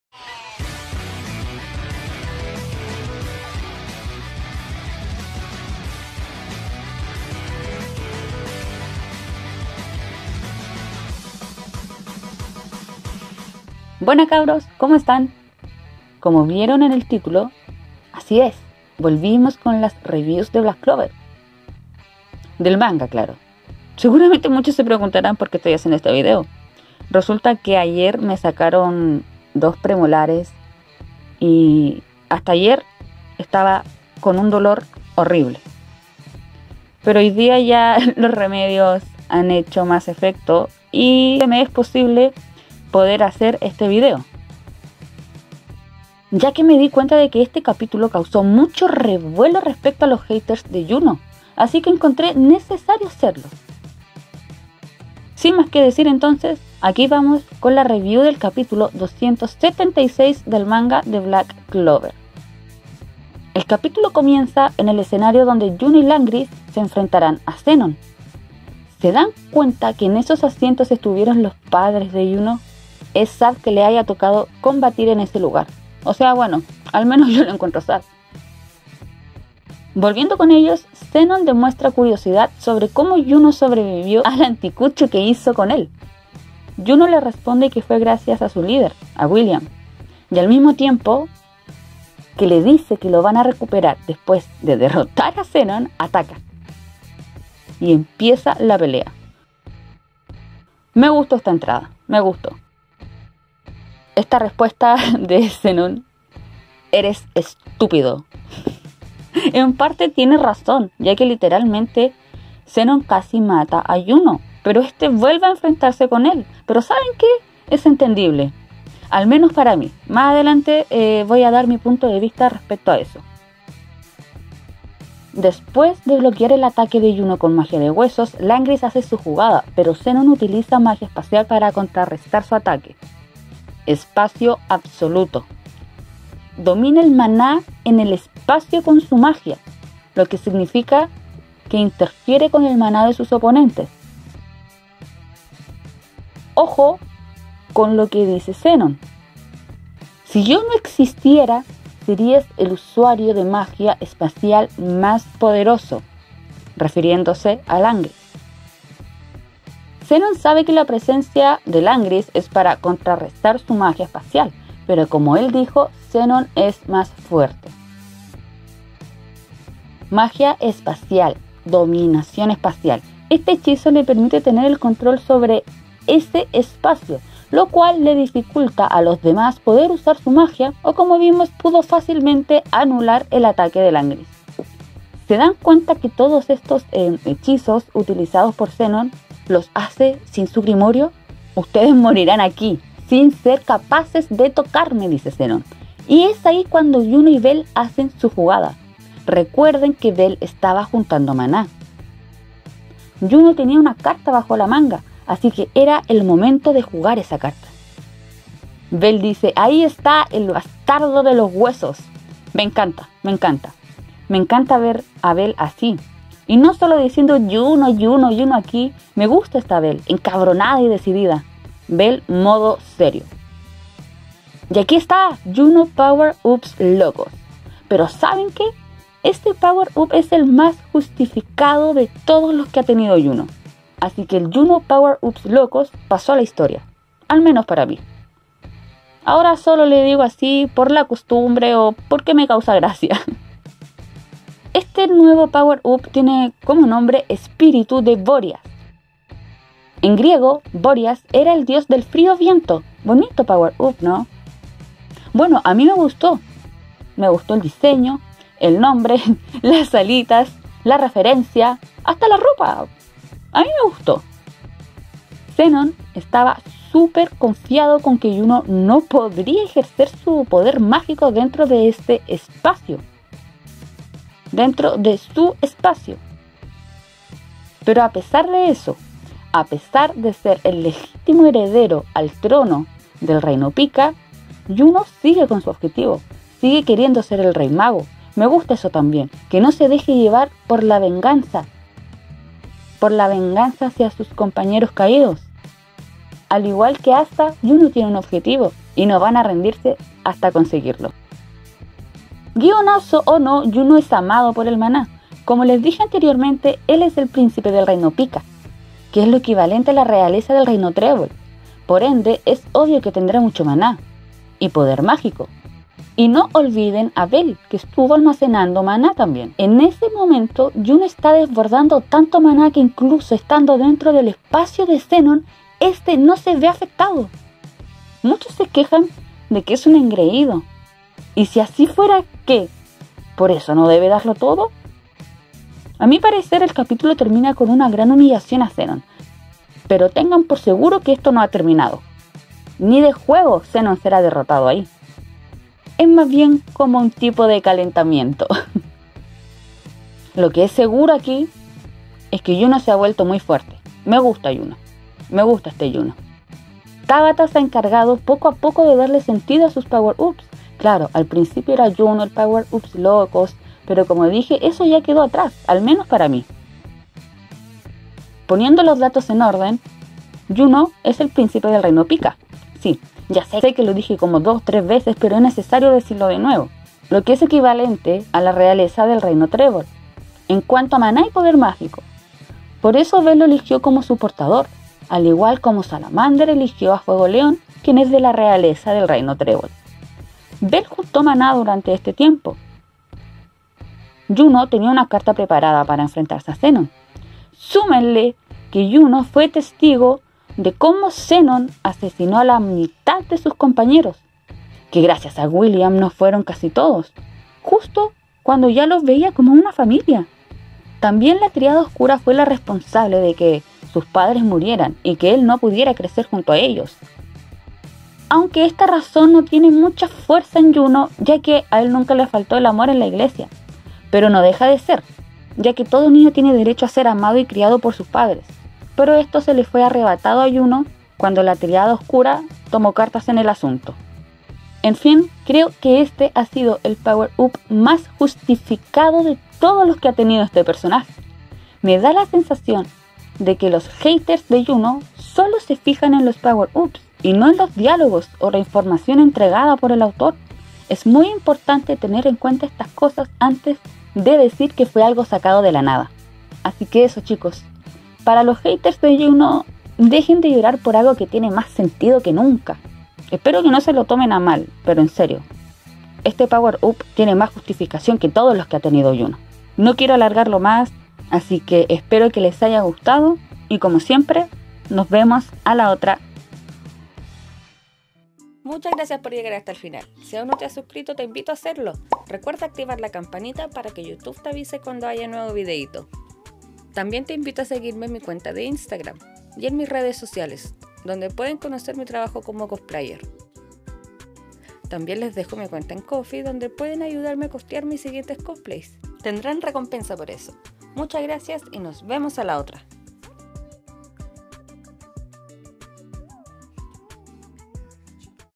YUNO POWER UPS LOCOS QUEDÓ ATRÁS!! | YUNO Y LANGRIS VS ZENON | REVIEW BLACK CLOVER | CAPITULO 276
Buena cabros!

YUNO POWER UPS LOCOS QUEDÓ ATRÁS!
ME GUSTÓ MUCHO COMO SE DESARROLLO ESTE POWER UP, NO ENTIENDO POR QUÉ ALGUNOS LE TIRAN HATE u-u

QUE ME DICEN USTEDES? LES GUSTÓ ESTE NUEVO PODER DE YUNO? CREEN QUE SEA SUFICIENTE PARA DERROTAR A ZENON?

• Les gustó la miniatura? Es trabajo de

LAS IMAGENES UTILIZADAS EN LA MINIATURA SON COLOREOS DE FANS CON SUS RESPECTIVAS FIRMAS! LOS DERECHOS A ELLOS UWU

Espero les guste y nos vemos a la otra ❤️